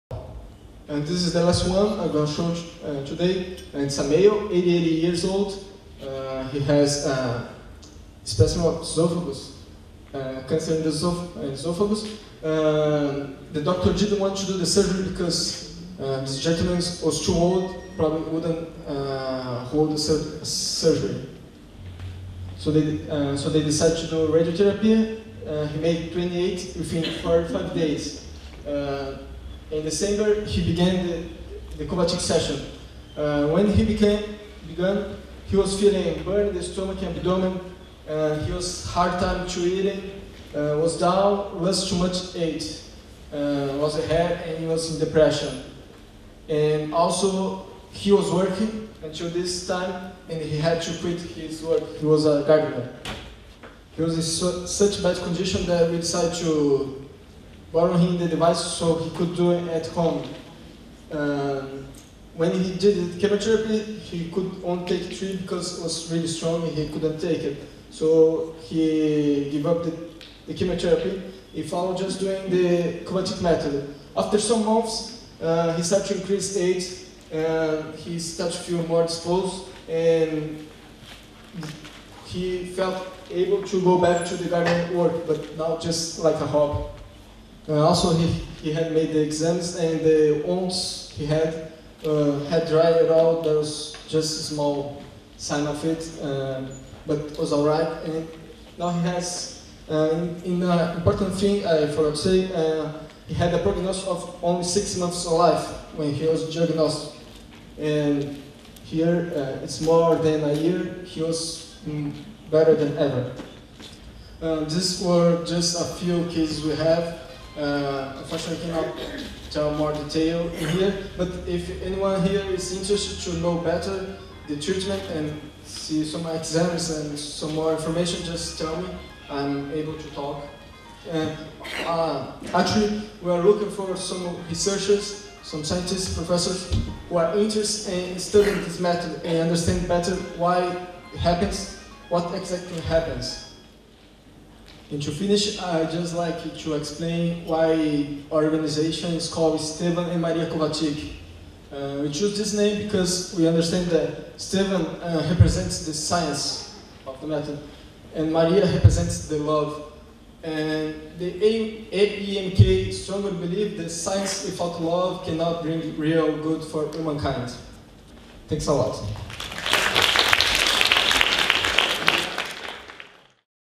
4/4 - Lecture about Kovacsik Method for cancer by Daniel Kovacsik from AEMK - English/Português
4/4 - Palestra sobre o Método Kovacsik para câncer por Daniel Kovacsik da Associação Estevam e Maria Kovacsik - São Petersburgo/Russia - 04/07/10

Lecture about Kovacsik Method for cancer by Daniel Kovacsik from Associação Estevam e Maria Kovacsik - St Petersburg/Russia - July 4, 2010.

English/Português.